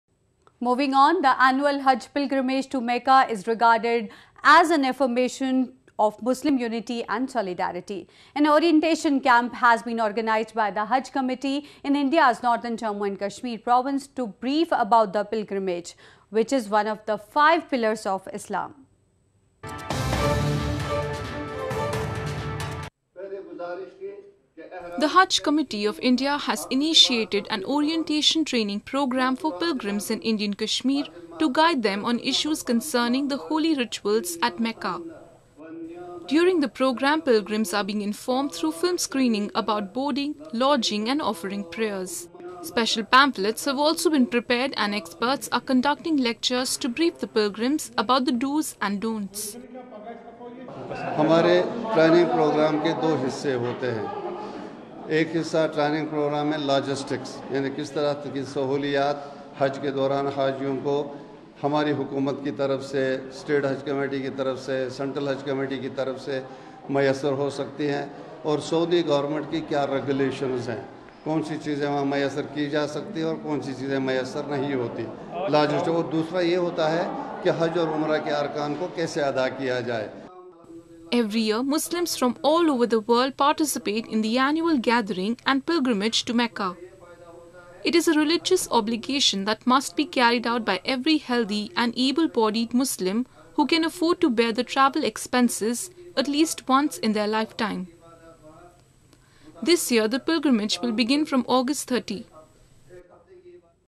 India News (16 Jun, 2017) - Haj Committee organizes orientation camp for pilgrims in Indian Kashmir
The annual Haj pilgrimage to Mecca is regarded as an affirmation of Muslim unity and solidarity. An orientation camp has been organized by the Haj Committee in Indian Kashmir to brief about the pilgrimage, which is one of the five pillars of Islam.